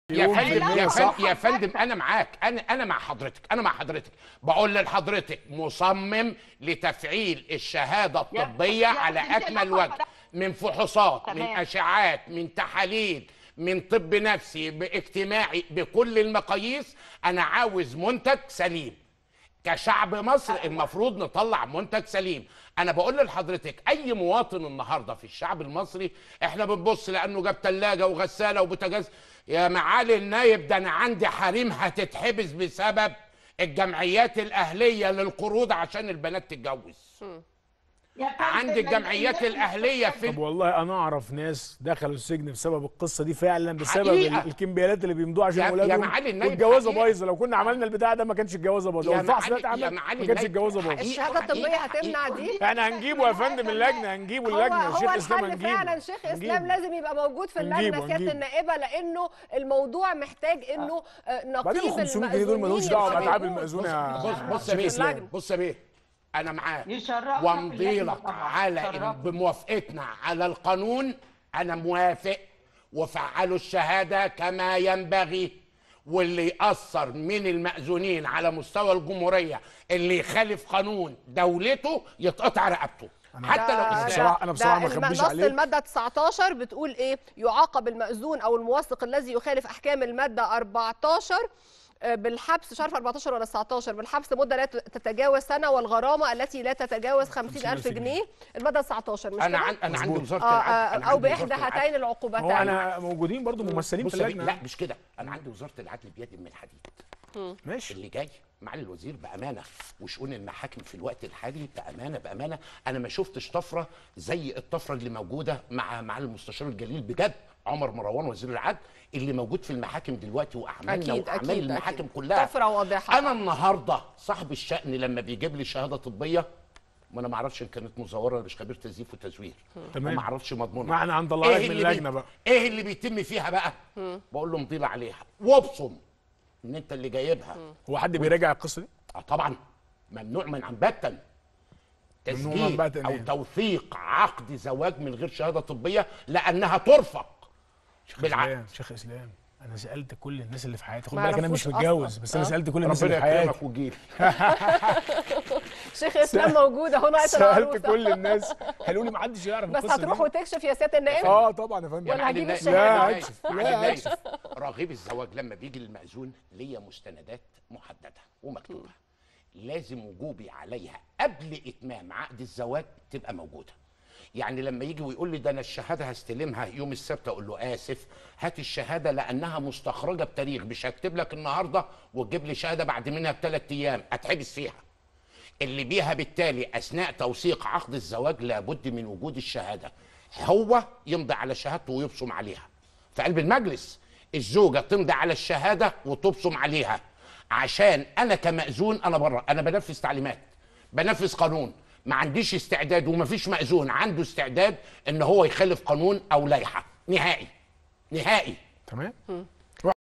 النائب كريم السادات: الكمبيالات اللي بيمضيها أهل العروسين لتجهيز الأبناء السبب الرئيسي في فشل الزواج
وتضمن مشروع القانون عددا من الإجراءات الطبية والنفسية والاجتماعية التي يقوم بها المقبيلين على الزواج، بلإضافة إلى الإلزام بعمل التحاليل والفحوصات المختبرية والأشعة التشخيصية، كما يستهدف الأمراض الوراثية والمناعية والمعدية بالإضافة إلى الفحص الشامل للذكورة والنساء والصحة النفسية.